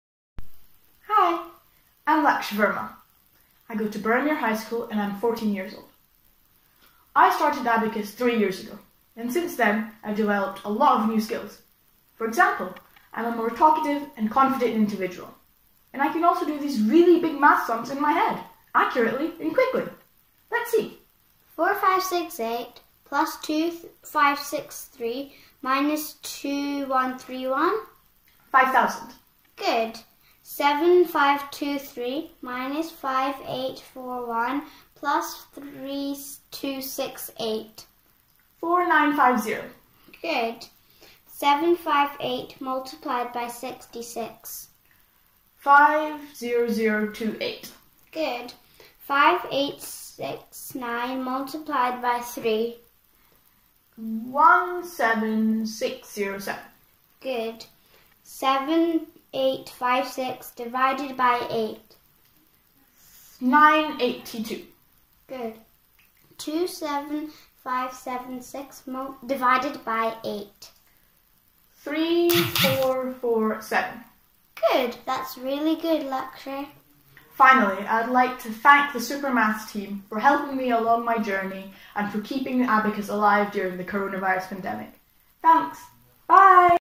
How to Learn Abacus Maths Online with Fun? | BYITC
Lakshya Verma takes every Maths challenge passionately. He is a hardworking, passionate and competent learner and as a result, he builds a better understanding of the Abacus Mental Maths Theory. He thinks it’s fun and easy to solve calculation by the Abacus method.

Walkthrough of the World’s First Abacus Maths E-learning Web Application

for registering your interest in the online classes.